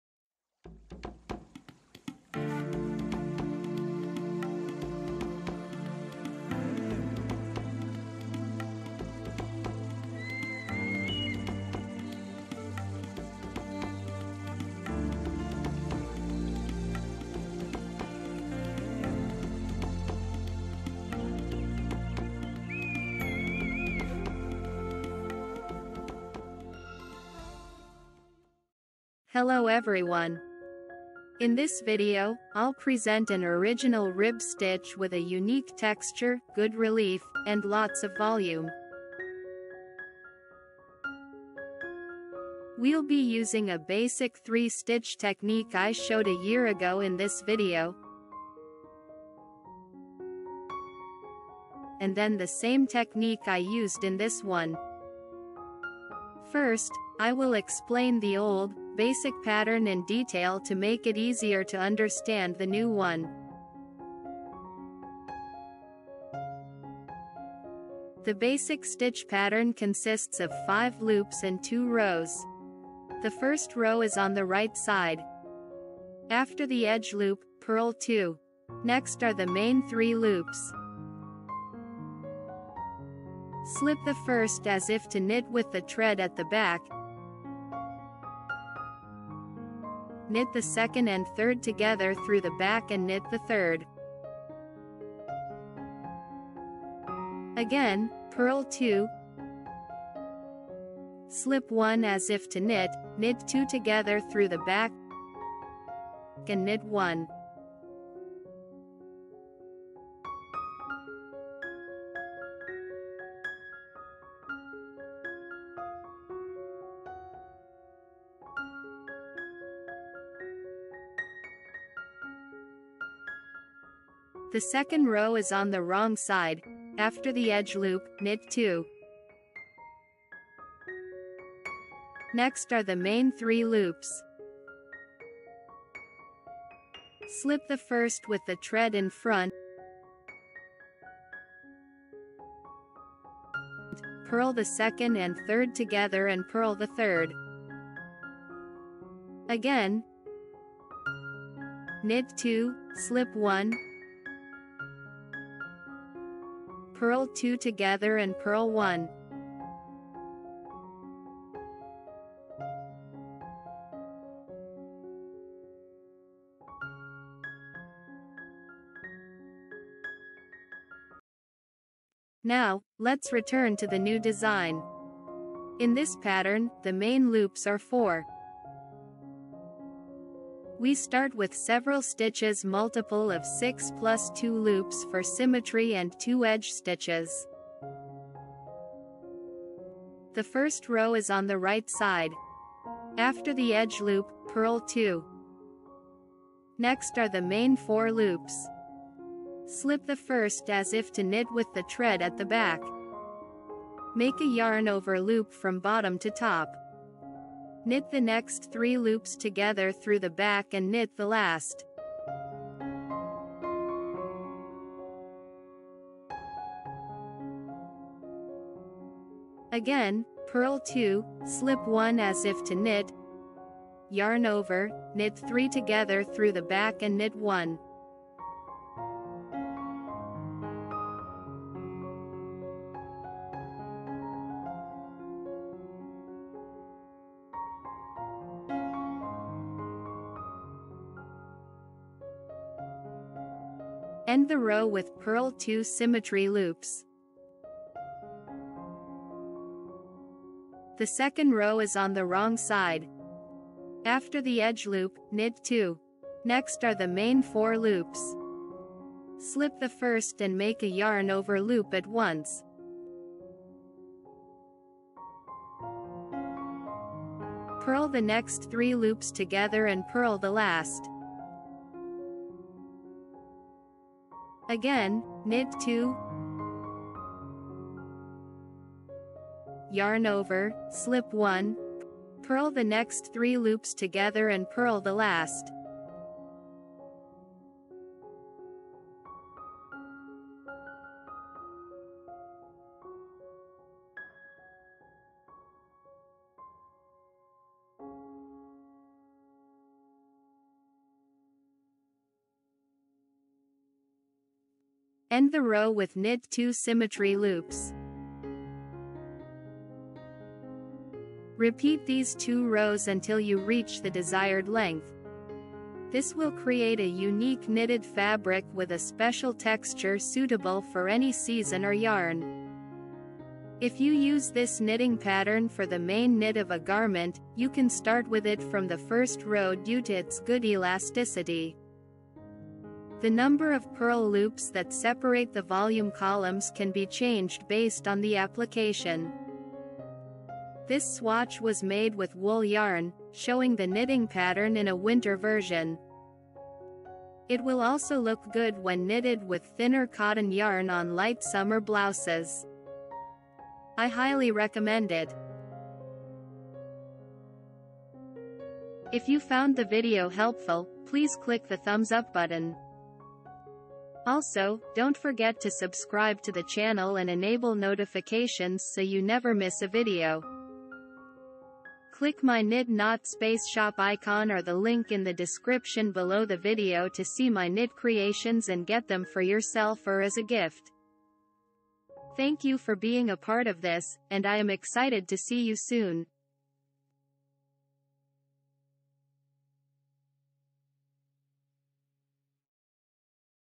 [English] Wide-Chain Rib Stitch. An extremely elastic and embossed knitting pattern.
This unique knit creates a knitted fabric with a special texture suitable for any season or yarn. If you use this knitting pattern for the main knit of a garment, you can start with it from the first row due to its good elasticity. The number of purl loops that separate the volume columns can be changed based on the application. This swatch was made with wool yarn, showing the knitting pattern in a winter version. It will also look good when knitted with thinner cotton yarn on light summer blouses. I highly recommend it.

00:00 Promo
03:10 Row 1
04:10 Row 2
05:10 Conclusion.

If you'd like to check out my knit creations and get them for yourself or as a gift,